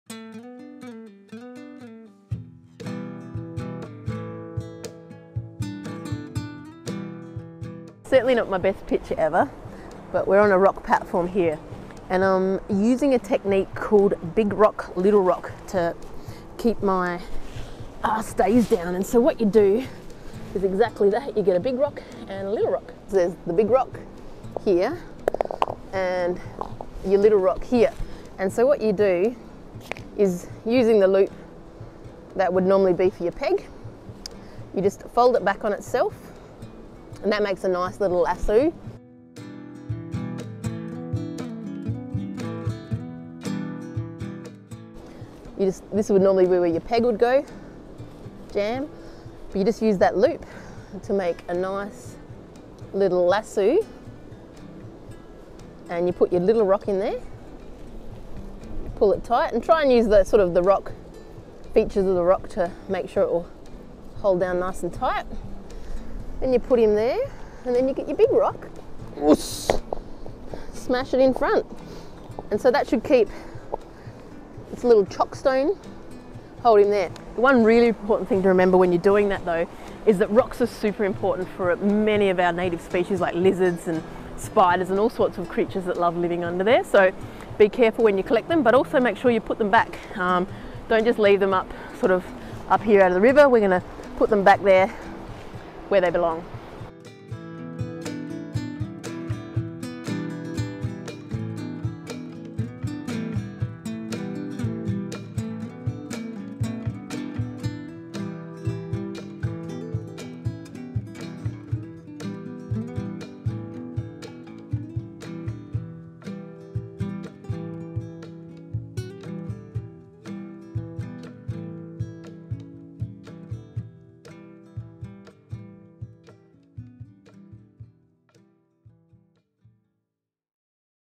Big Rock Little Rock | How to pitch a tent without pegs/stakes on rocky ground
This video shows you how to set up your tent without pegs

Many tents these days require a good pitch to keep them standing especially many ultralight tents like my Zpacks duplex

But often the coolest places to camp don’t have beautiful soft ground to put pegs into. I used this method on many Australian hiking trails like the Larapinta, Jatbula, cape to cape and when wild camping off track in the budawangs. The best views are often rocky platforms. These campsites, up high are some of the most spectacular, so this method is great for allowing you to camp safely and still get amazing views.

Staking out your tent safely can be done using the Big Rock Little Rock method. The first thing you need is a length of string coming off of your tent tie down points. You can use para cord or a shoelace. I leave these on my tents at all times to help with this method if I can’t get pegs in.

I like to use a bowline knot to create the loop at the end. You then loop it back on its self making a little loop to catch the small rock. Once secure plop a big rock in front of the small rock - to create a chock stone with the small rock. Repeat for each tie down point. In really windy weather make sure you have a good amount of weight for the big rock.

Always return rocks after using them as they are important habitat for our native species. Also be careful collecting rocks as snakes and spiders may be under them.

I hope this helps you stake out your tent out on rocky ground.

Let me know if you have any tips or tricks for pitching in areas where you can’t get a peg in the ground.

If rocks are limited try something similar with perhaps branches (small branch, big branch)